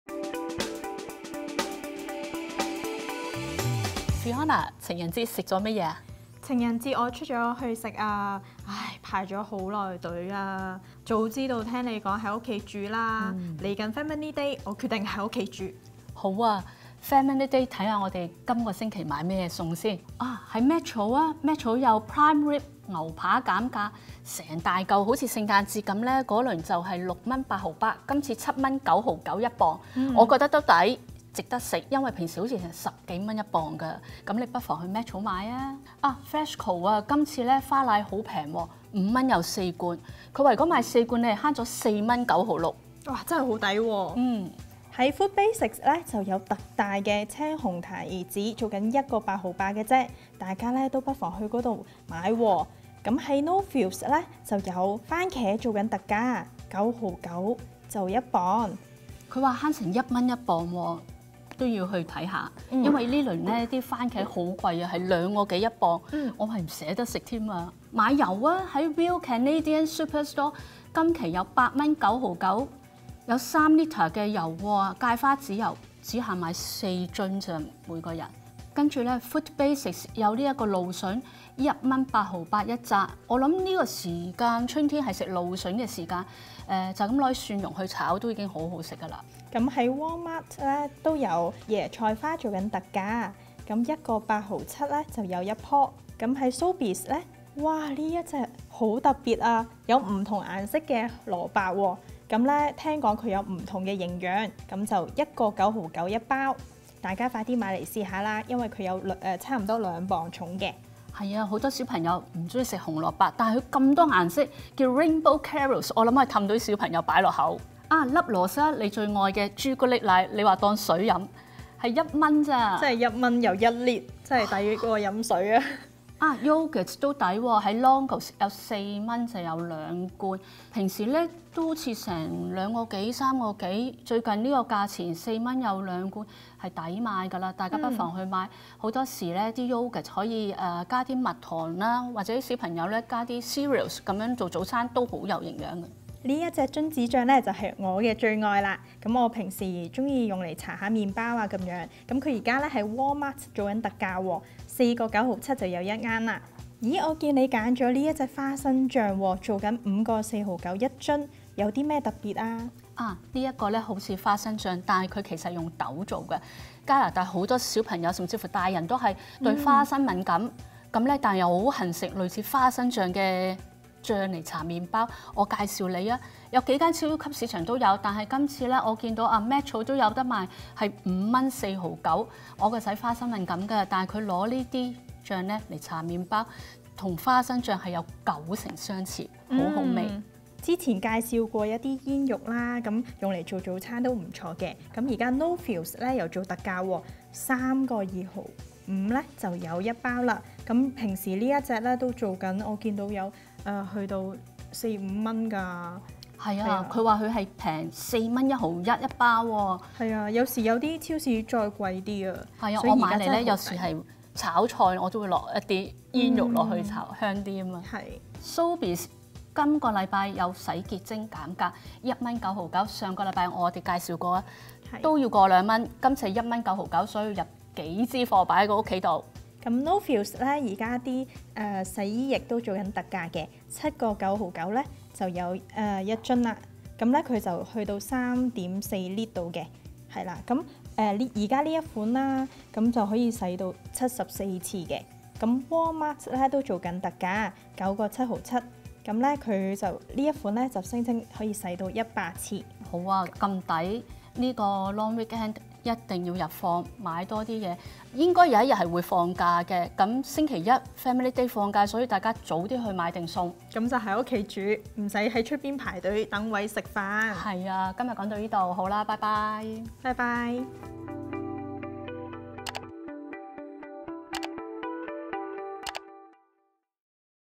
CCUE Flyer指南｜牛肋扒特價家庭日煮大餐 番茄近期最抵9毫9一磅 煙肉平過半價 4公升洗衣液激減$7.7 超市特價 #20230216
【精明消費】各大超市每星期特價產品多得令人眼花繚亂，究竟Flyer上的食物是否真的抵買？作為精明消費者，最好「慳得一蚊得一蚊」，CCUE請來多倫多黎太廚房的黎太，每星期幫大家選出當期最優惠食材，並提供烹調方法建議，務求慳到盡！

今期精選特價有：
牛肋扒
三花淡奶
提子
番茄
芥花籽油
紅蔥
椰菜花
各色蘿蔔
朱古力奶
乳酪
榛子醬
豆製仿花生醬
煙肉
洗潔精
洗衣液

本周Flyer：2023年2月16日 - 2月22日